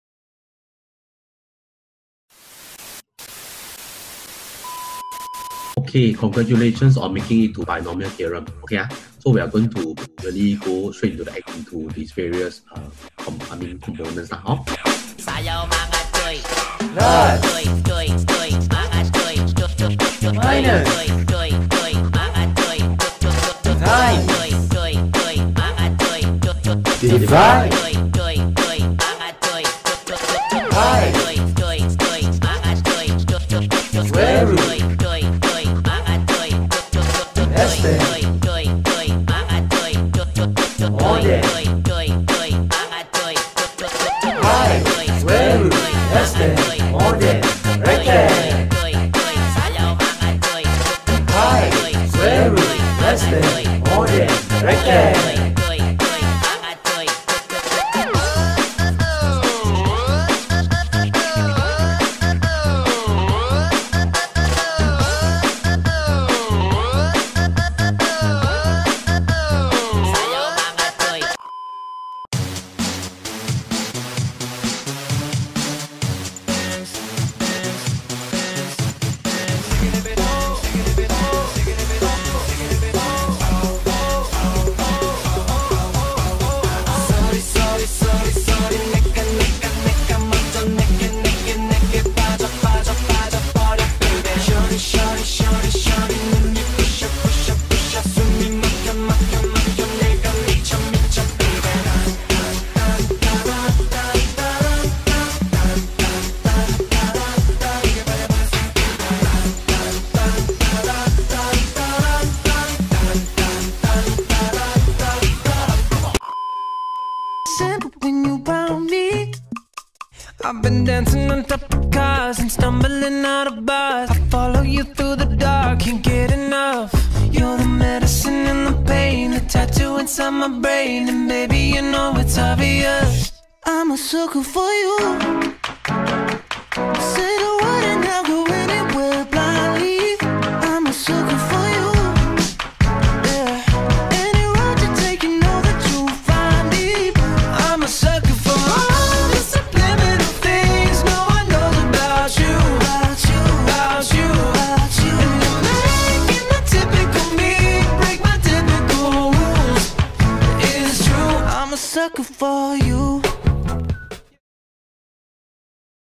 NJ Experience Episode 3: NJ’s Got Talent (Arcadance)
Curious to know more about the NJ experience? Come take a look at our very own talented NJCians showcasing their singing and dancing skills!

Do head over to our live Q&A sessions streamed on YouTube and Facebook on 9th January and 12th January from 10am to 1pm to find out more!

Facebook - National Junior College Singapore